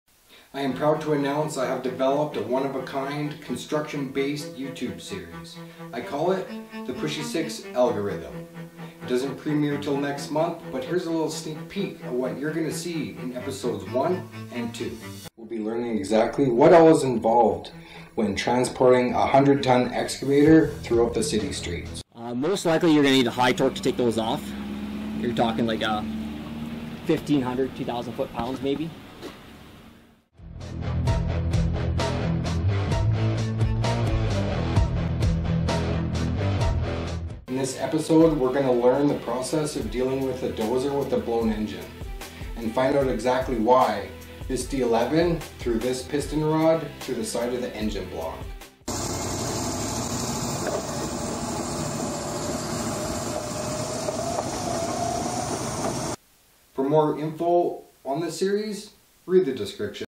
The Pushysix Algorithm Series Sneak Peek
Every episode(15 minutes) covers a new construction topic, giving the viewer an
in-depth multi perspective of the process from start to finish like never done before...
Some of the processes you will learn about are:
-heavy haul transport -dealing with broke down equipment
-equipment maintenance
-site planning and  management
-job estimating and bidding
-becoming a heavy equipment operator with advice and tools to succeed
-utilizing equipment and training operators to increase production
-mass excavation
-underground utilities
-client relations -industry safety
-equipment rentals vs purchasing
Series will also include product reviews, deleted scenes/bloopers, and Q&A's.
Expect exclusive interviews from many construction professionals from operators all of the way up to company owners.
1st episode premieres on the pushysix youtube channel in march 2018